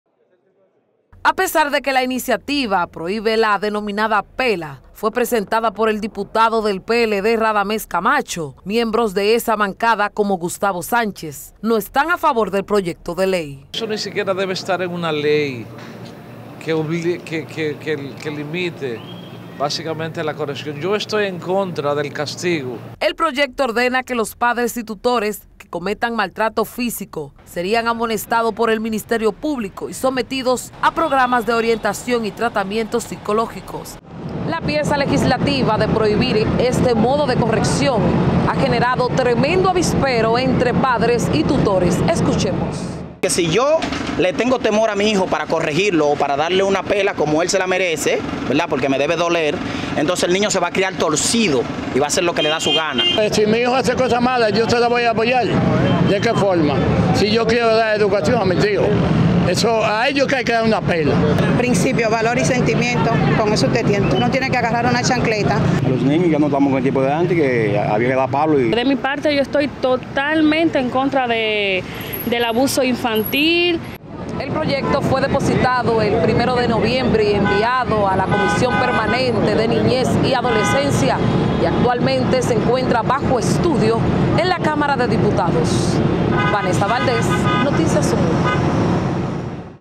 “A ellos hay que darle una pela” Ciudadanos reaccionan ante proyecto de ley prohibirá “pela”
| Canal 101 de Claro, 1101 Claro HD, Canal 20 de Telecable Dominicano y 46 de Wind Telecom.